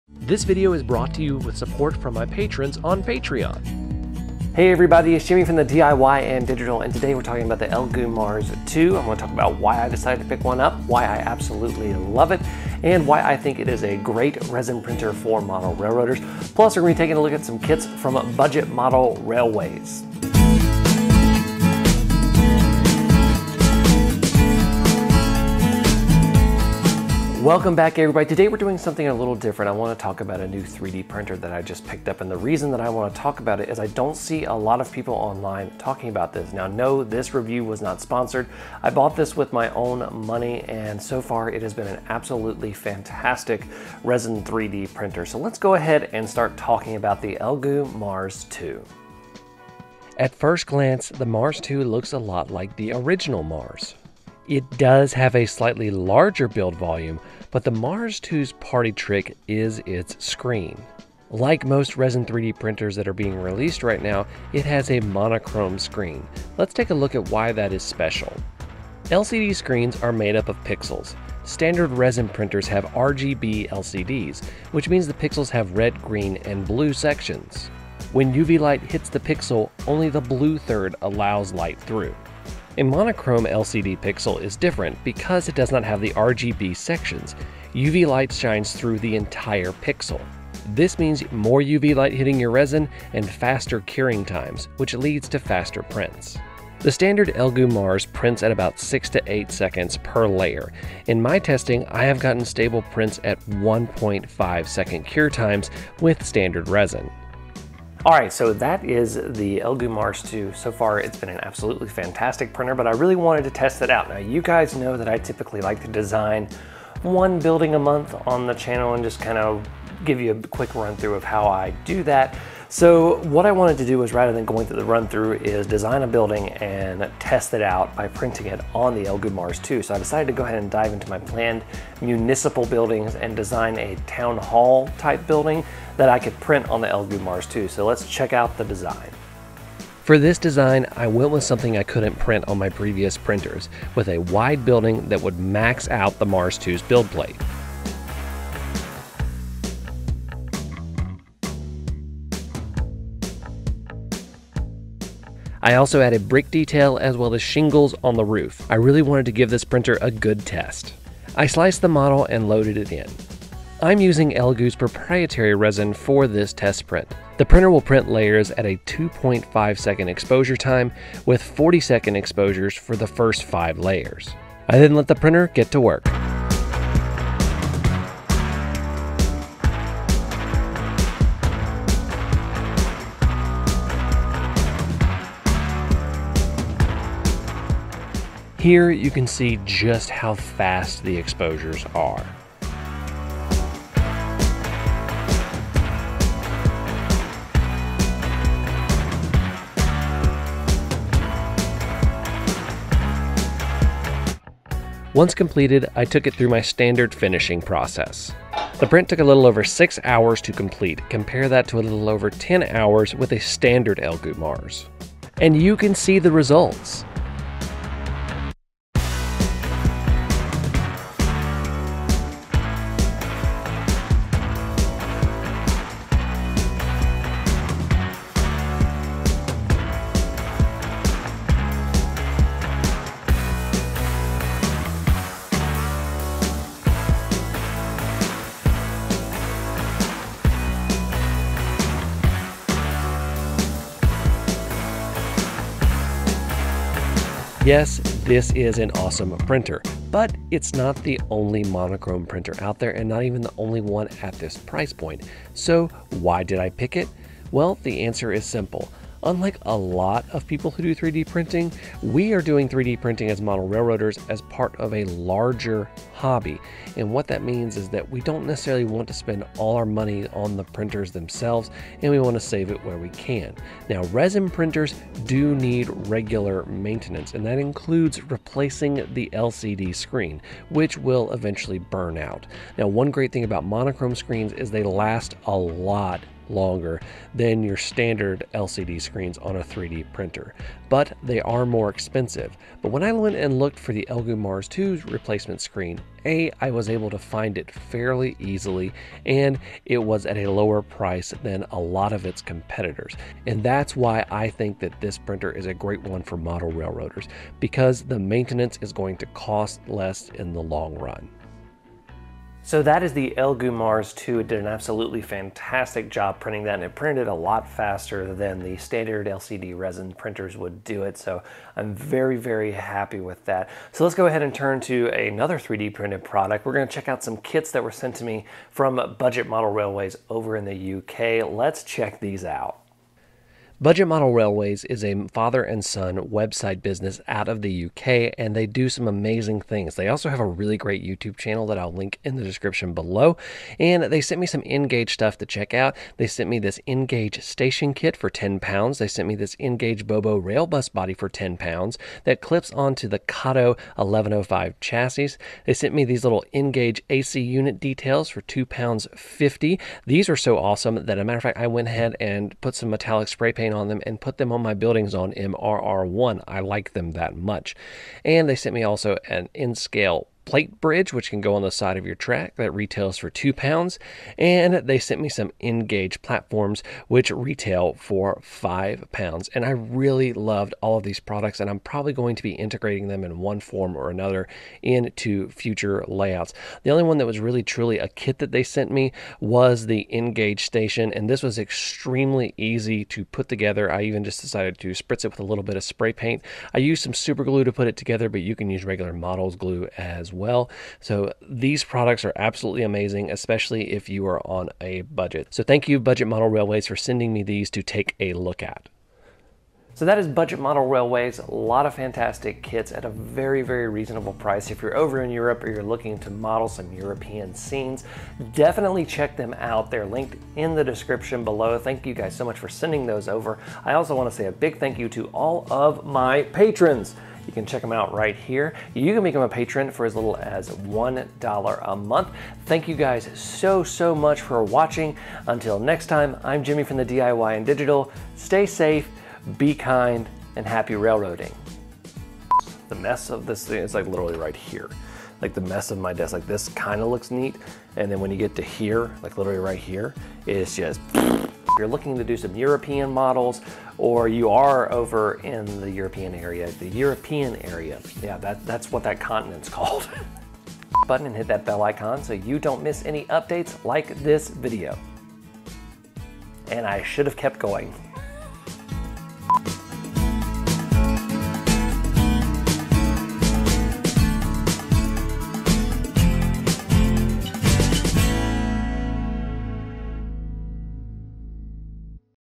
A Budget Friendly 3D Printer for Model Railroaders: A Review of the Elegoo Mars 2
Welcome back everybody!  Today I am talking about a new 3D Printer that is out that I absolutely love and it is perfect for model railroading, especially if you are looking to get into 3D Printing.  The Elegoo Mars 2.  This is a simple, and budget friendly resin 3D printer with a monochrome screen that allows for faster prints over previous generations.  I am also taking a look at some products from Budget Model Railways!

Elegoo Mars 2

Elegoo Resin

I do a lot of 3D printing

Printers Used

Elegoo Mars 2

Camera Used